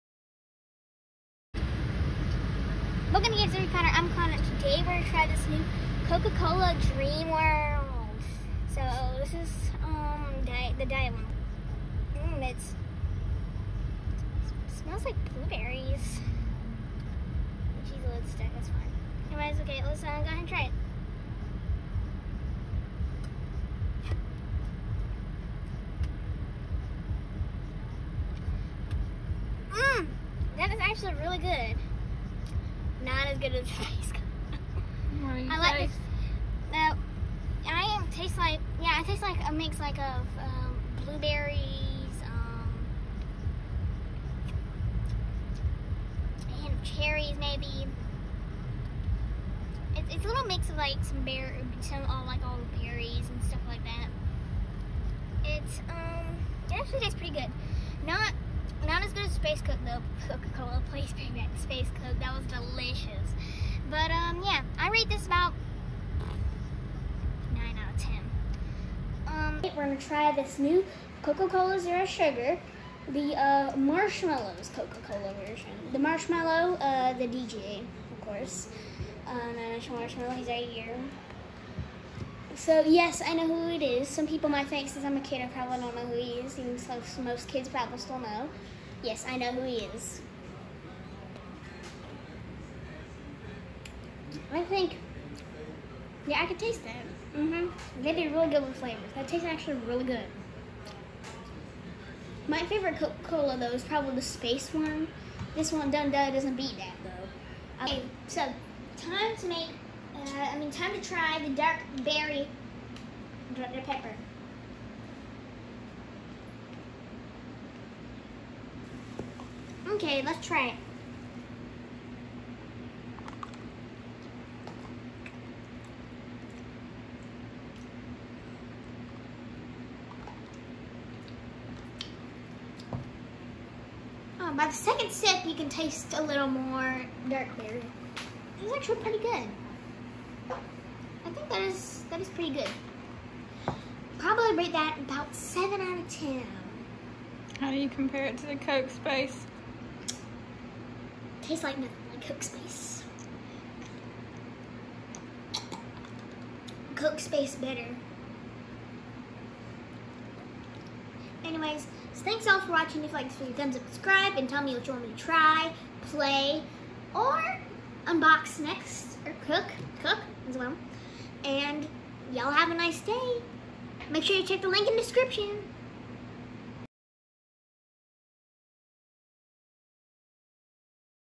SODA TASTING / DREAM COKE / MARSHMALLOW COKE / DARK BERYY DR PEPPER
A compilation of soda tasting